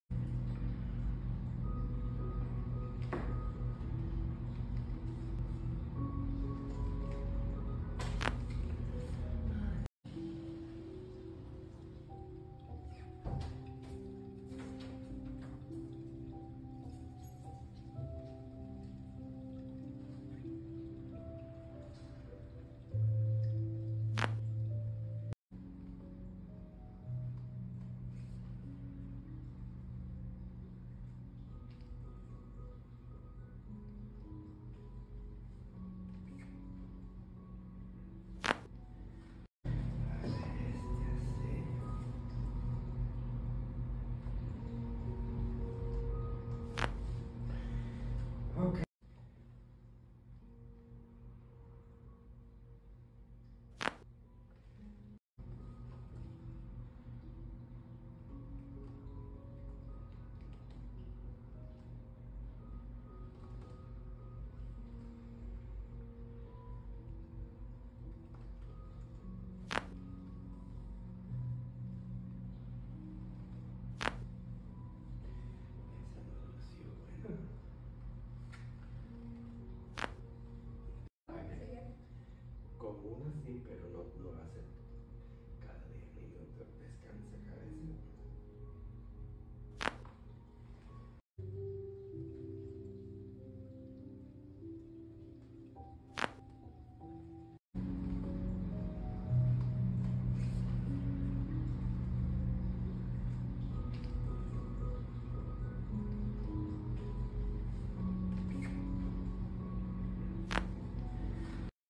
Escucha el CRUJIDO: Sonidos de Ajustes Quiroprácticos
🖐 En este video, te invitamos a disfrutar de una experiencia única llena de sonidos satisfactorios que acompañan los ajustes quiroprácticos. 👀 Playlist de Ajustes

Sumérgete en el mundo de la quiropráctica mientras escuchas los crujidos que se producen durante cada ajuste, diseñados para aliviar la tensión y restaurar la alineación en la columna vertebral. Estos sonidos no solo son fascinantes, sino que también reflejan la liberación de presión y el bienestar que se siente después de un tratamiento.

Perfecto para quienes buscan relajarse o simplemente disfrutar de los efectos terapéuticos del ASMR, este video te permitirá conectar con tu cuerpo y experimentar la tranquilidad que ofrecen los ajustes quiroprácticos.